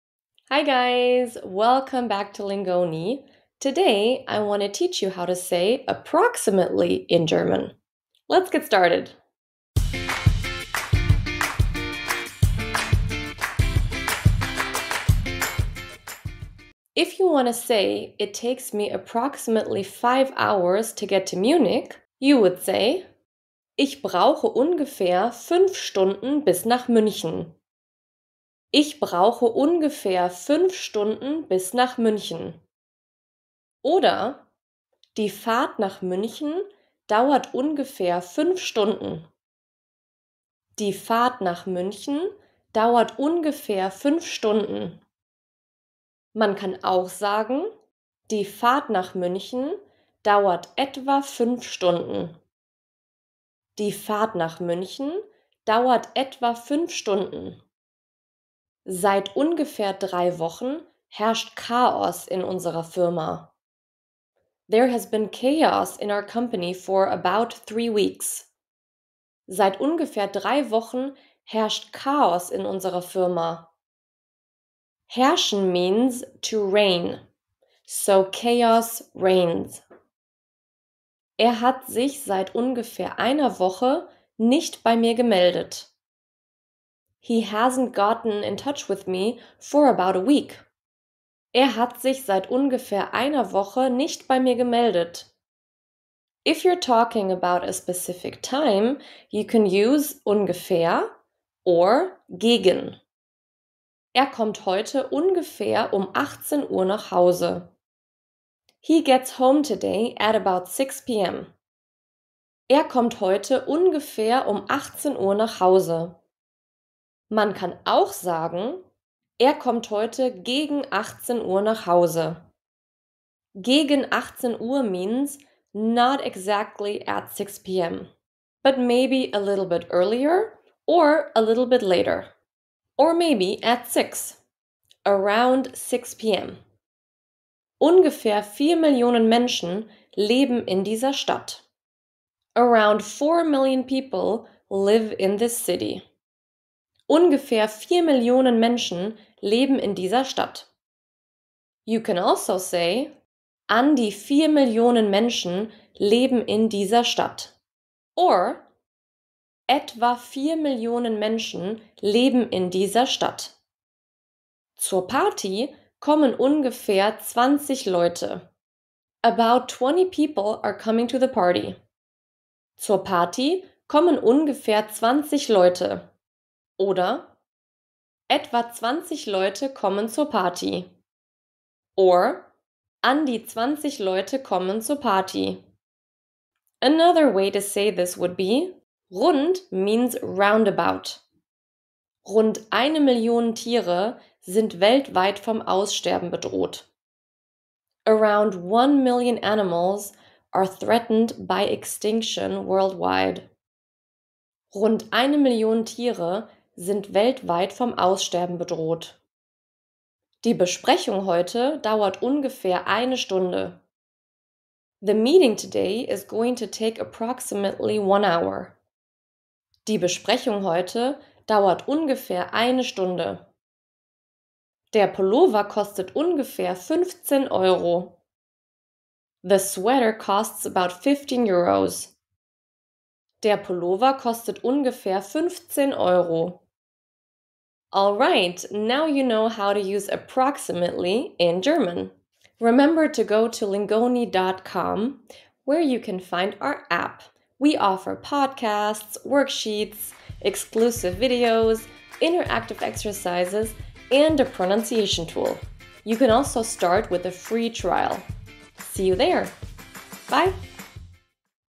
How to say APPROXIMATELY in German - A2
Are you curious about how to say "approximately" in German? The most common word Germans use is "ungefähr", but there are quite a few alternatives as well!

and discover how efficient and fun language learning can be.

Improve your German reading comprehension
A1/A2 Books
B1/B2 Books

00:00 Intro
00:23 ungefähr
00:46 etwa
01:59 gegen
02:33 an die
03:11 rund